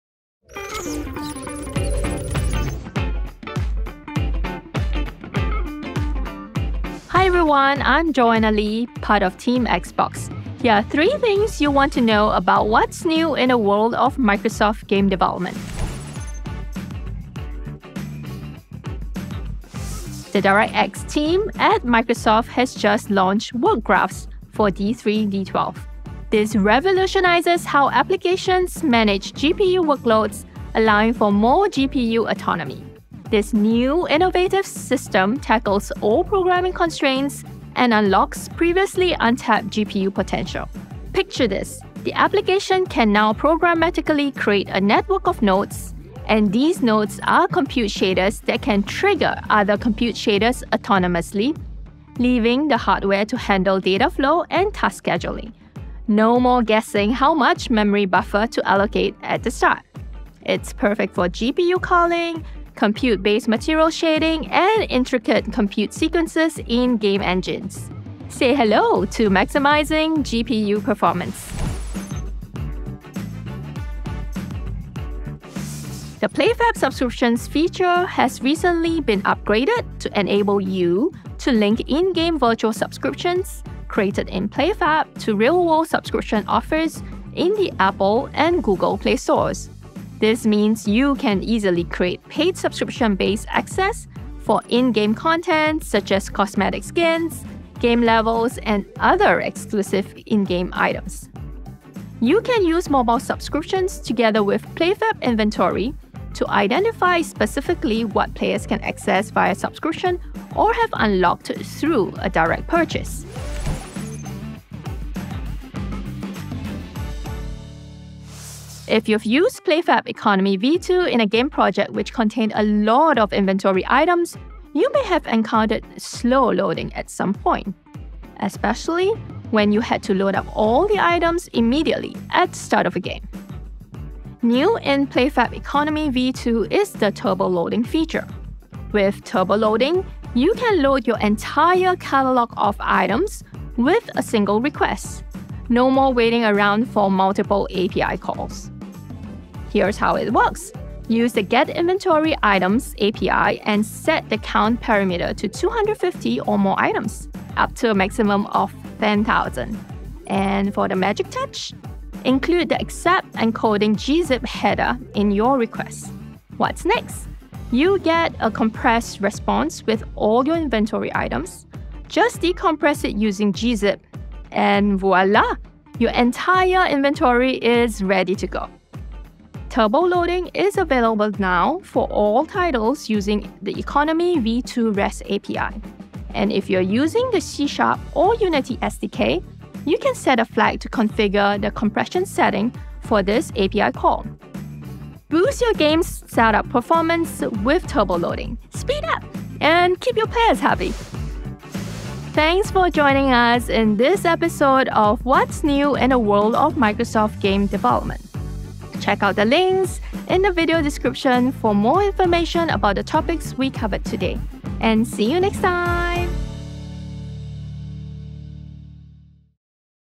What's New in Microsoft Game Development
Discover the latest in Microsoft game development - Work Graphs in D3D12 for optimized GPU performance, PlayFab Mobile Subscriptions that link in-game content to real-world offers, and PlayFab Economy v2 Turboloading to dramatically speed up game startup times. Elevate your game development with these new features!